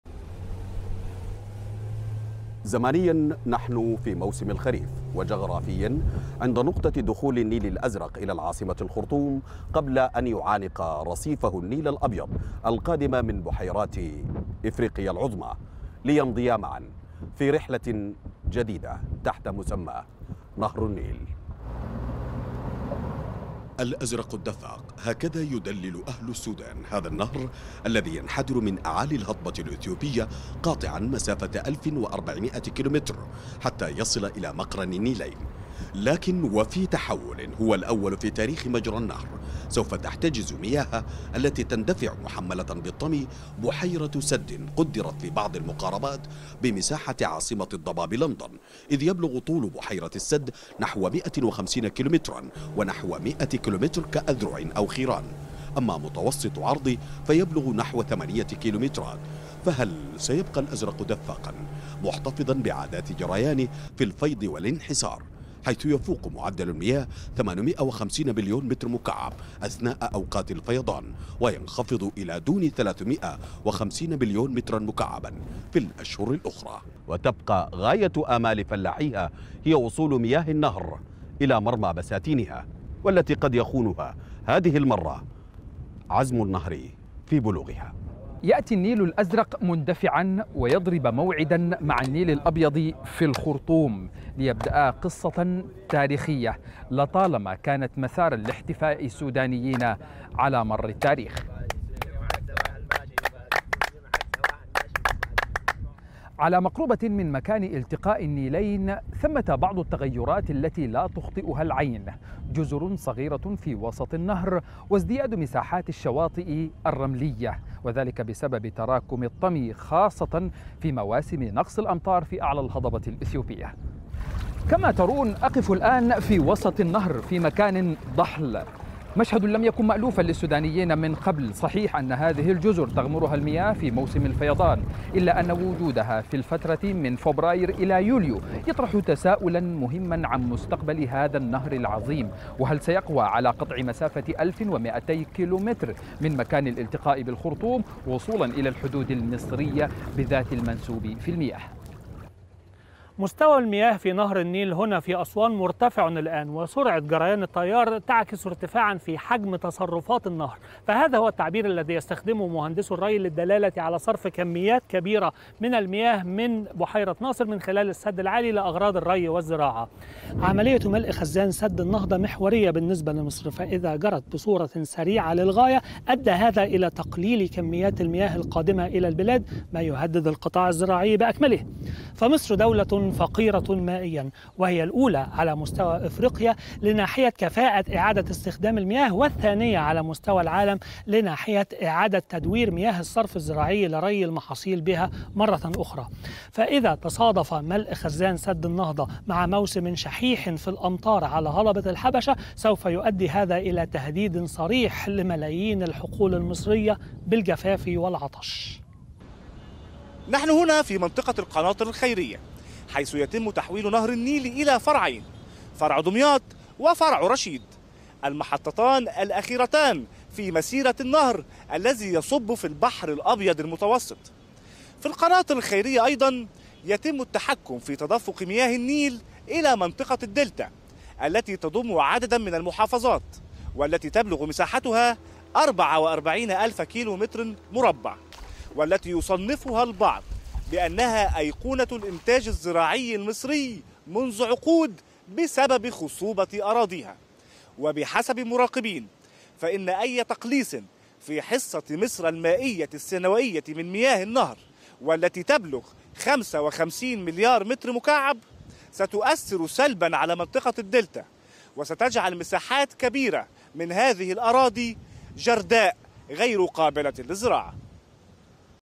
"الحدث" تسرد قصة نهر النيل "أبو الحياة" من المنبع إلى المصب
4 مراسلين من مواقع مختلفة في السودان و مصر.. الحدث تسرد قصة نهر_النيل "أبو الحياة" من المنبع إلى المصب